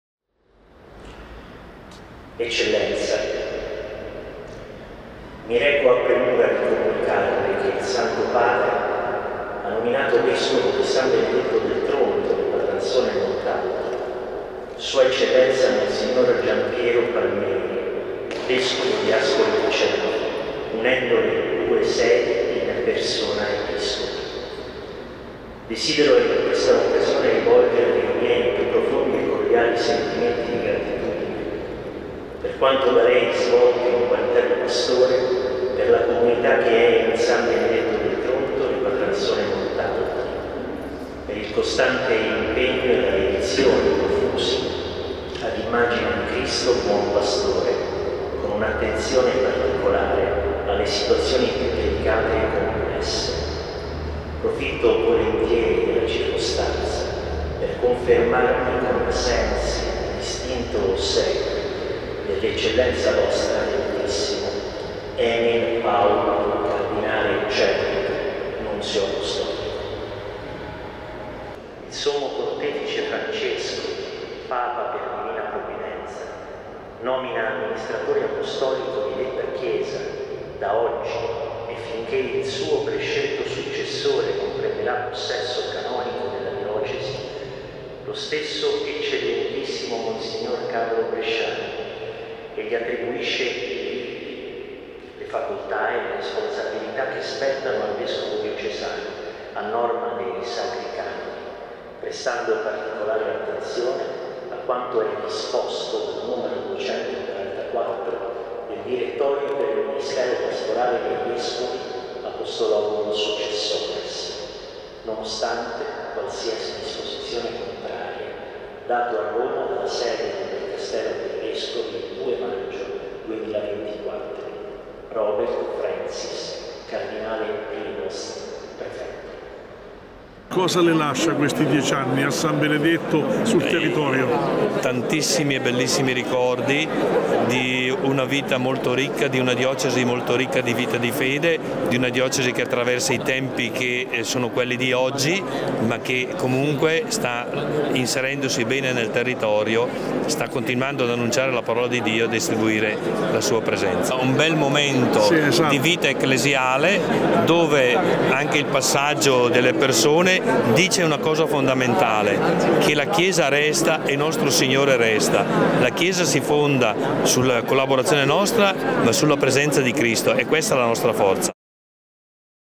Jant.it | Mons. Giampiero Palmieri è il nuovo Vescovo della Diocesi di San Benedetto del Tronto
Nel pomeriggio di domenica 30 giugno 2024, presso la cattedrale Madonna della Marina, Mons. Giampiero Palmieri si insedierà come nuovo Vescovo della Diocesi di San Benedetto del Tronto-Montalto e Ripatransone.